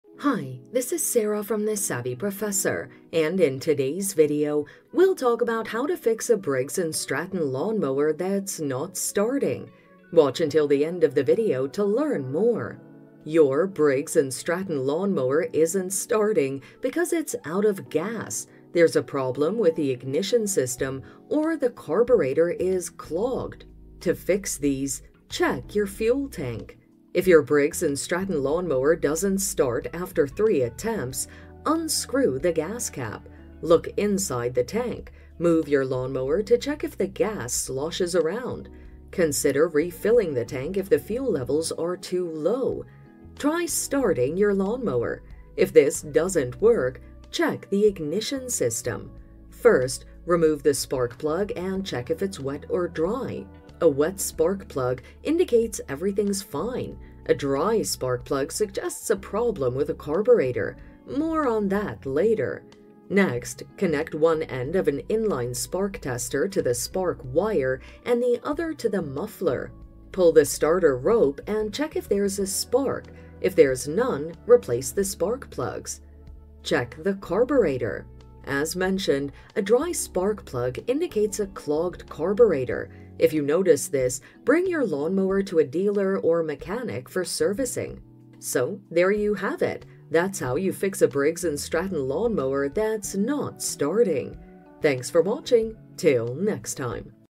How To Fix Briggs And Stratton Lawn Mower That's Not Starting (Simple Guide To Resolve The Issue!)
How To Fix Briggs And Stratton Lawn Mower That's Not Starting (Simple Guide To Resolve The Issue!). In this video tutorial I will show you how to fix Briggs and Stratton lawn mower that's not starting.

Your Briggs and Stratton lawn mower isn't starting because it's out of gas, there's a problem with the ignition system or the carburetor is clogged. To fix these,
Check your fuel tank.
If your Briggs and Stratton lawn mower doesn’t start after three attempts,
 Unscrew the gas cap.
 Look inside the tank. Move your lawn mower to check if the gas sloshes around.
 Consider refilling the tank if the fuel levels are too low.
 Try starting your lawn mower.

If this doesn’t work,
Check the ignition system.
 First, remove the spark plug and check if it's wet or dry. A wet spark plug indicates everything's fine; a dry spark plug suggests a problem with the carburetor... More on that later.
 Next, connect one end of an inline spark tester to the spark wire and the other to the muffler.
 Pull the starter rope and check if there’s a spark. If there’s none, replace the spark plugs.

Check the carburetor.
As mentioned, a dry spark plug indicates a clogged carburetor. If you notice this, bring your lawn mower to the dealer or mechanic for servicing.